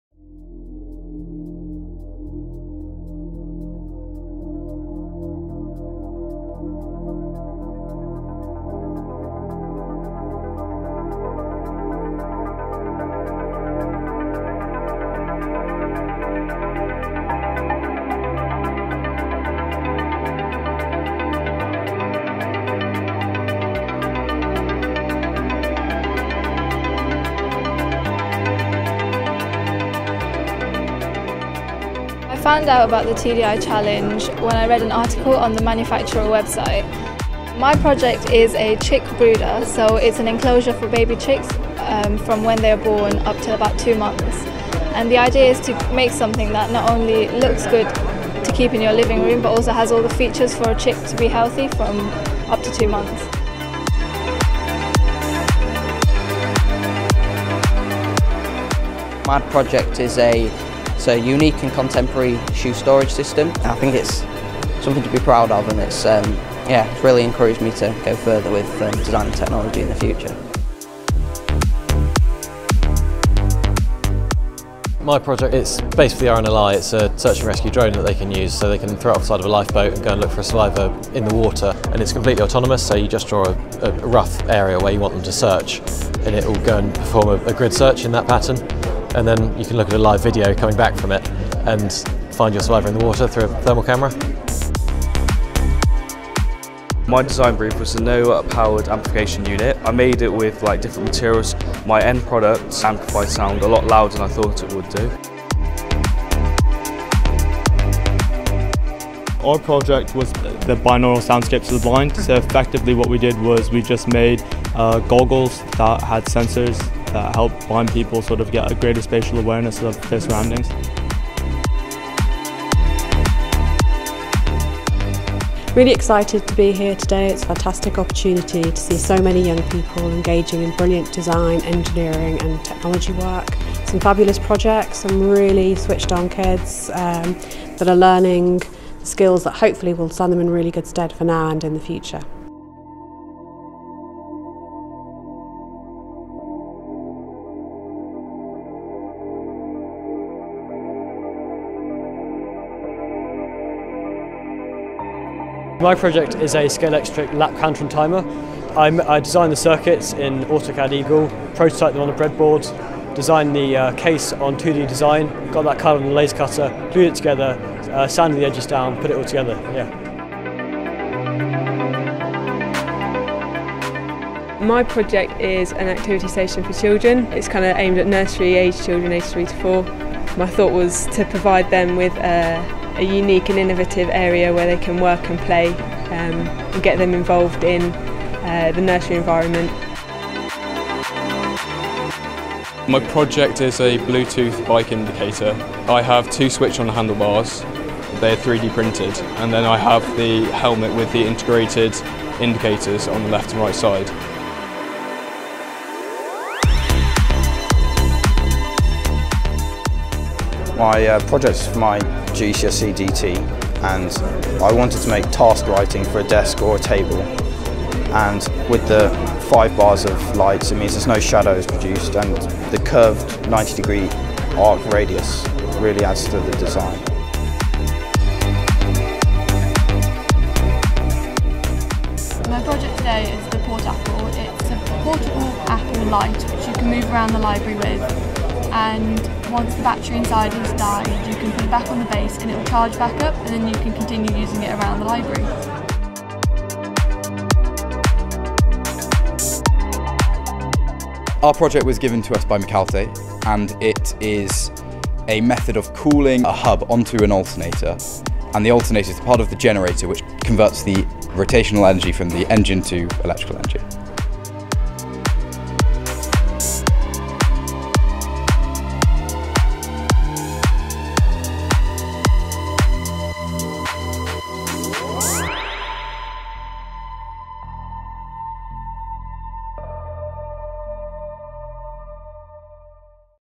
TDI Challenge 2017
The finalists of the TDI Challenge 2017 talk about their entries and the range of technologies and materials that were used.